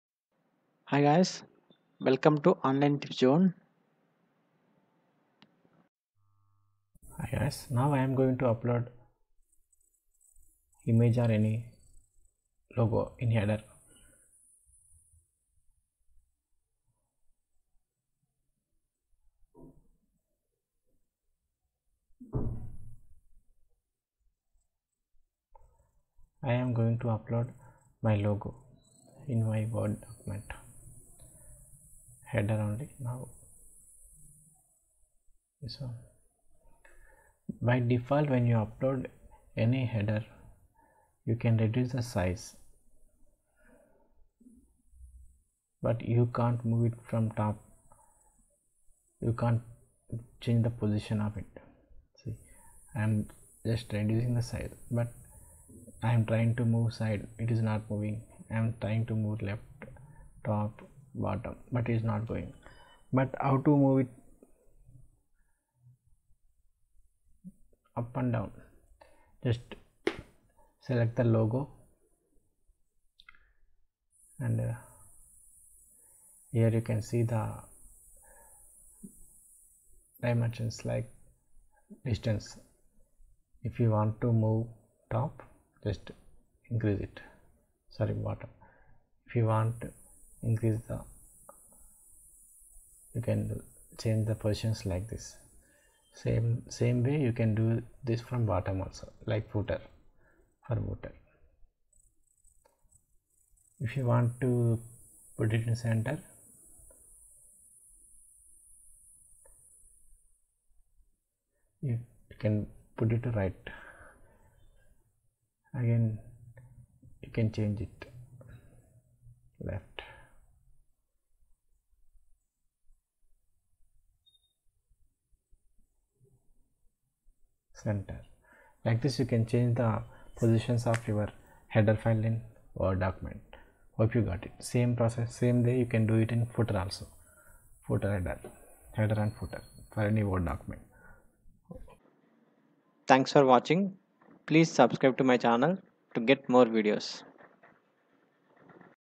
How to move Image in Word Header
We can move image or logo to anywhere in Word header and footers so that you can customize your word document with perfect header and footers